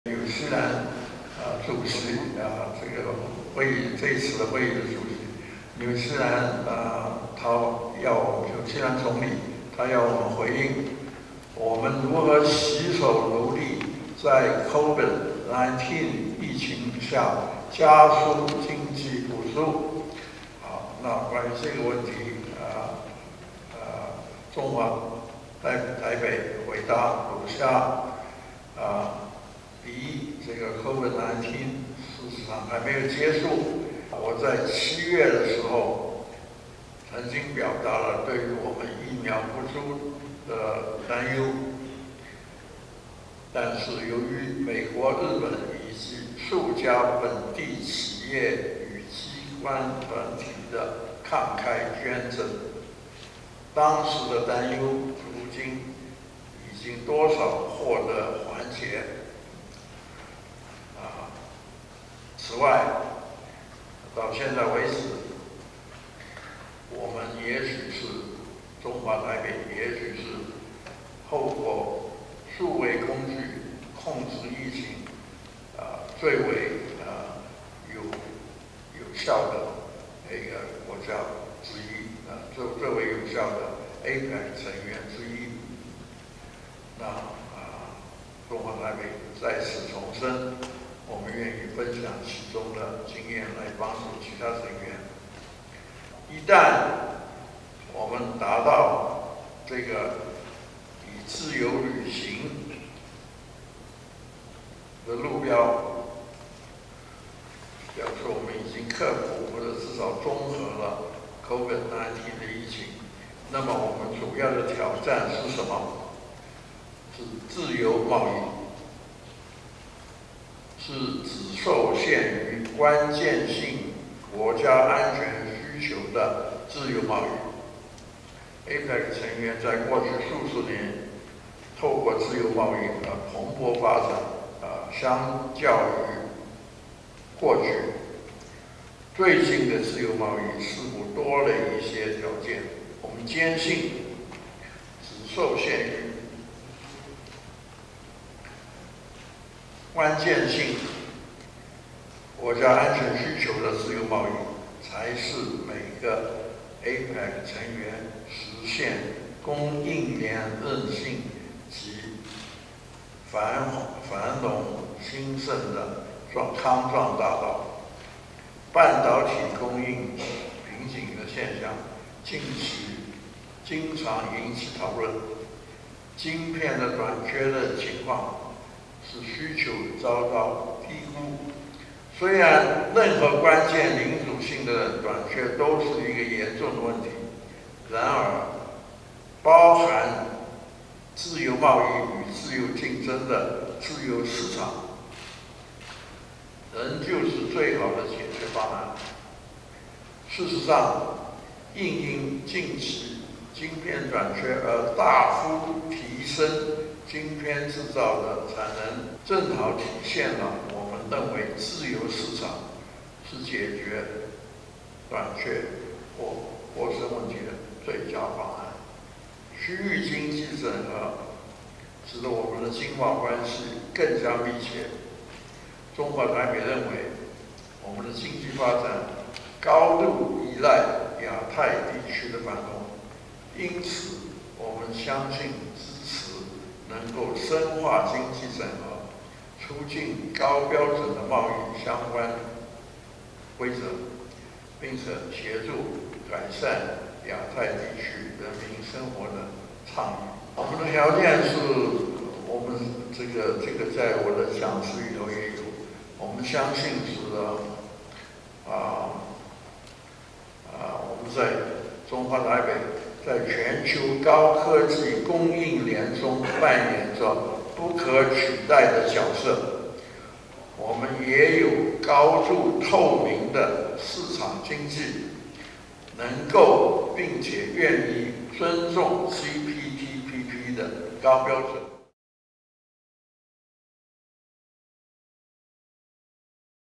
參加APEC爭取支持加入CPTPP 張忠謀：台灣在供應鏈中不可取代
有希望加入CPTPP 張忠謀：台灣條件相當好

由紐西蘭主辦的第29屆APEC「經濟領袖會議」昨晚7時以視訊會議舉行，9時45分圓滿結束。我國由台積電創辦人張忠謀擔任領袖代表全程與會，傳達我國理念與立場。張忠謀今天上午10時將在總統府召開記者會表示，我們已經加入申請CPTPP，一項可以為區域發展帶來動力的全面協定，在促進可持續成長的同時實現貿易和